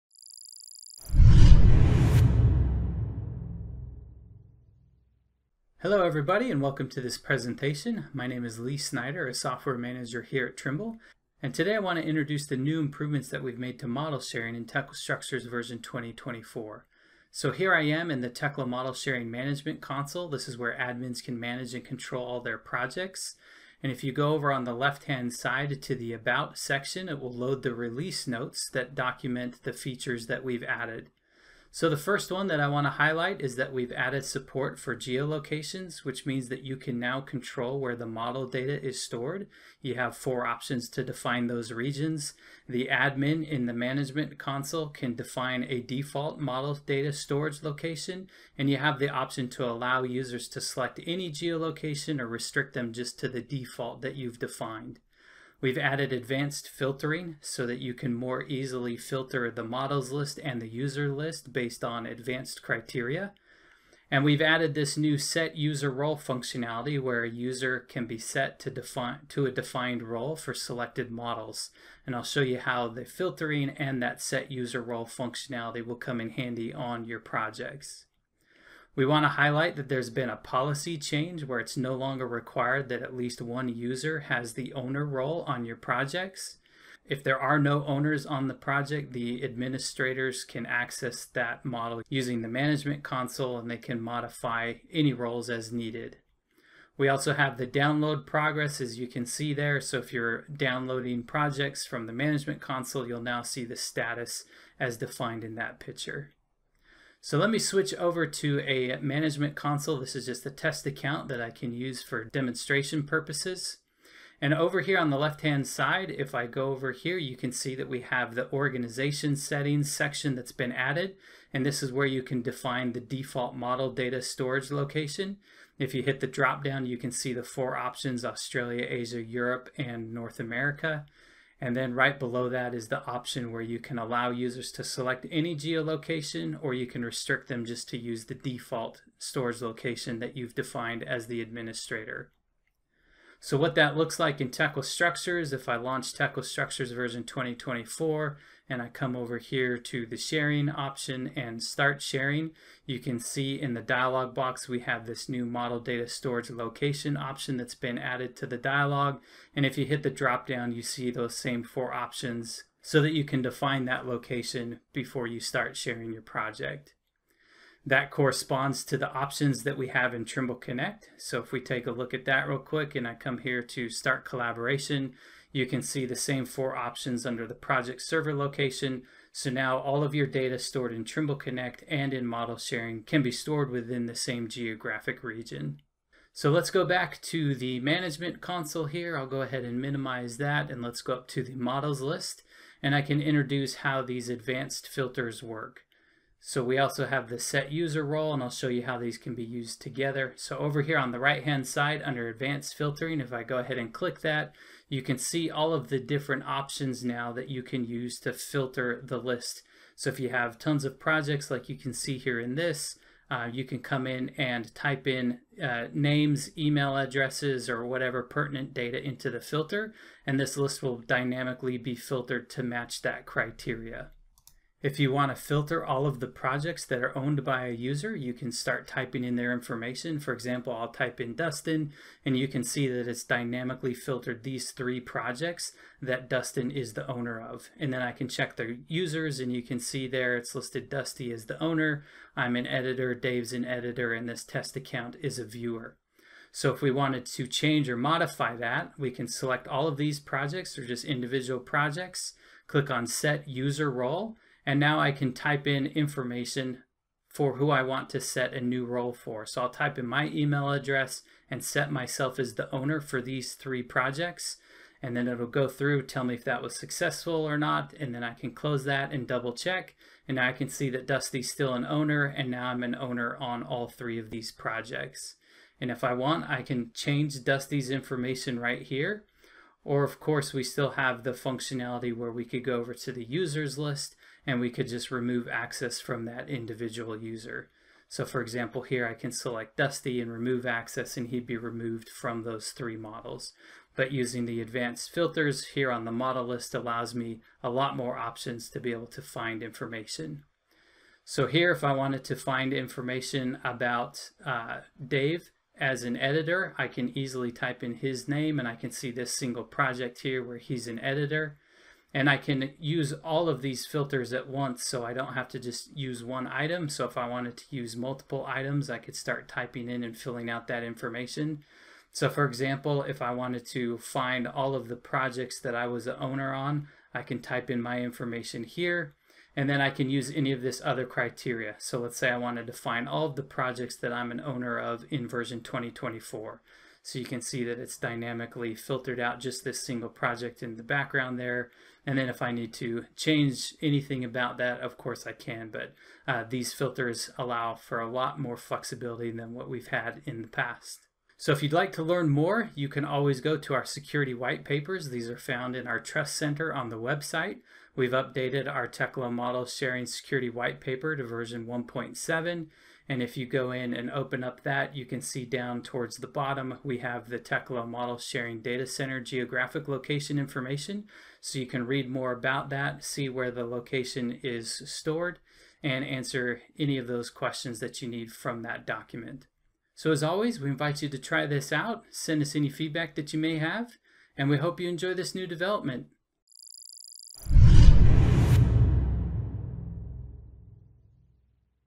Tekla Structures 2024 Tekla Model Sharing Improvements
This video features the main improvements to Tekla Model Sharing in Tekla Structures 2024 which include:
- Ability to select where your model data will be stored
- New advanced filtering capabilities in the Tekla Model Sharing Management Console
- New “Set user/role” command that enables you to set a user to a defined role for selected models
-New download progress status